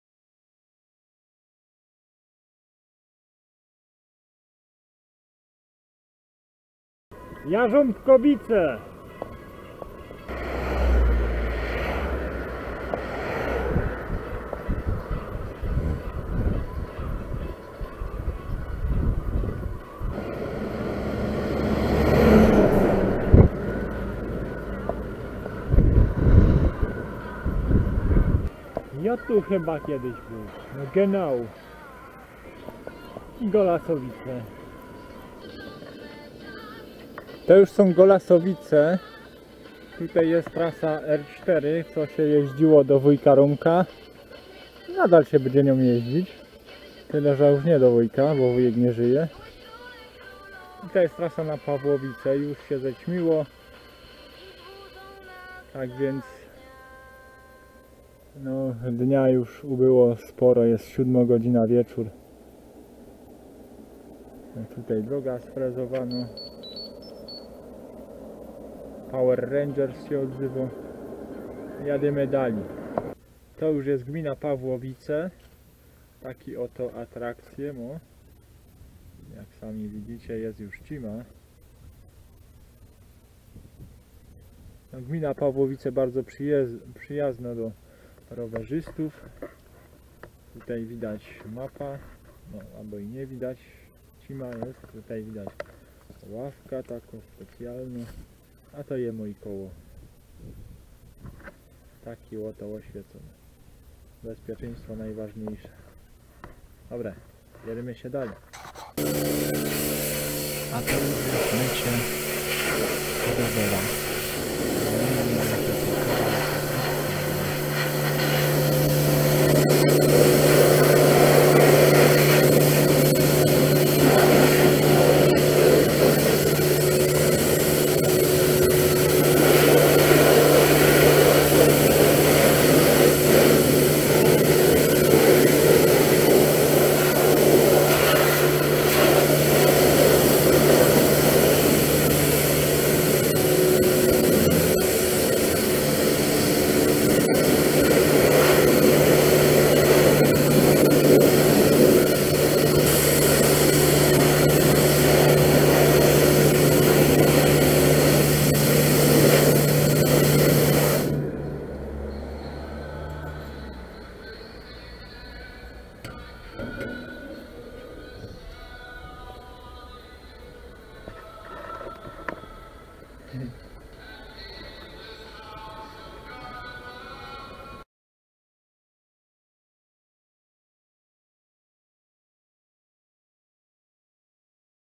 Gmina Pawłowice 16 10 2016 50km
Wyprawa rowerowa po gminie Pawłowice i okolicach. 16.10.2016 przejechane 50 km.